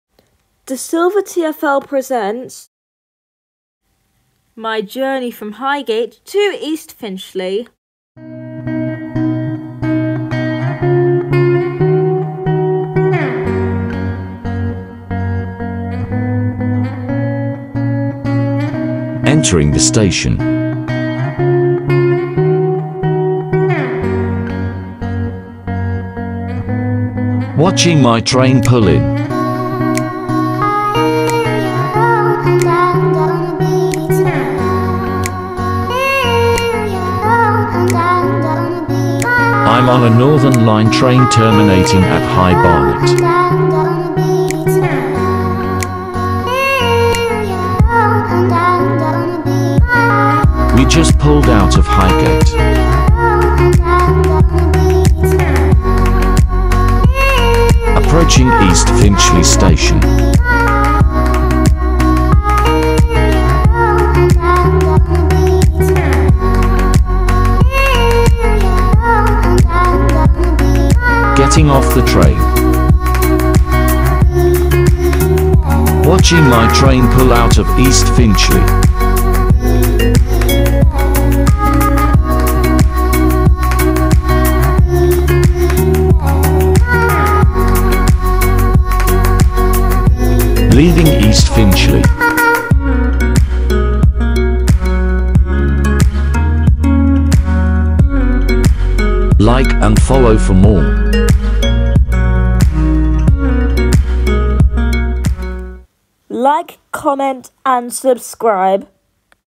My journey Highgate to East Finchley
A video of the journey from Highgate Station to East Finchley station. Complete with audio descriptions and subtitles.￼